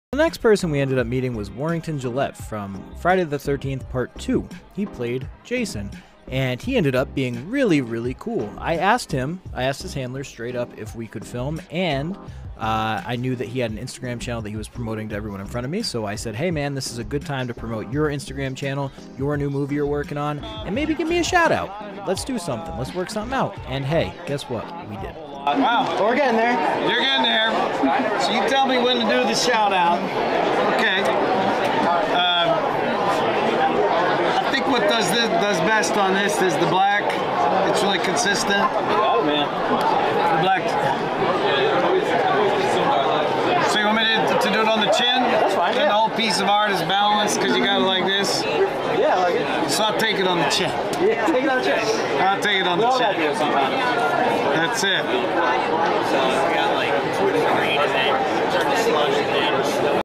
Warrington Gillette from Friday the 13th Part 2
Warrington Gillette talks to me about his new movie